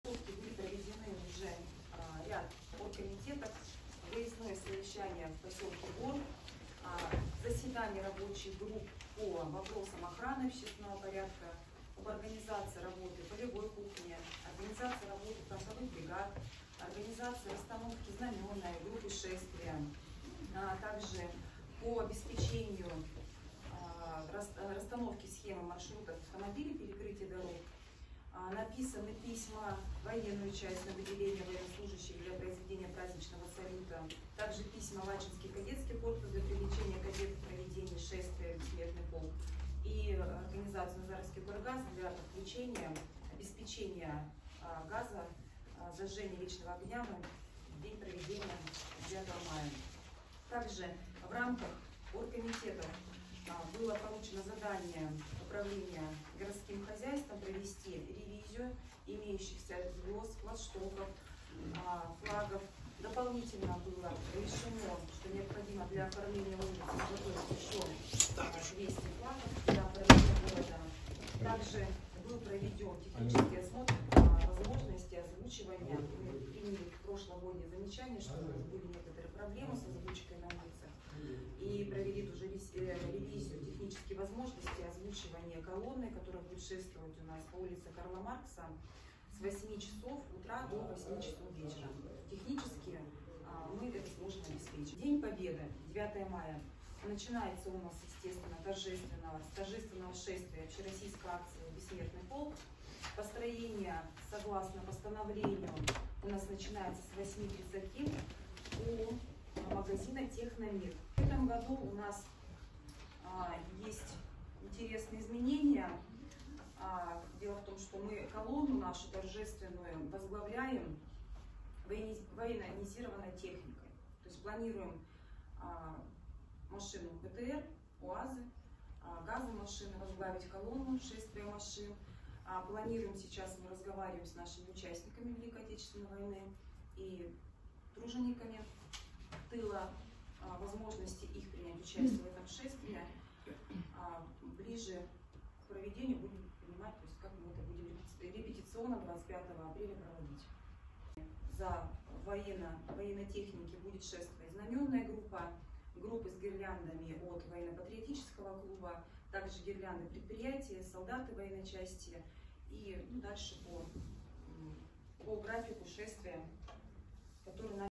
Ольга Туник
План мероприятий к 9 мая в Назарово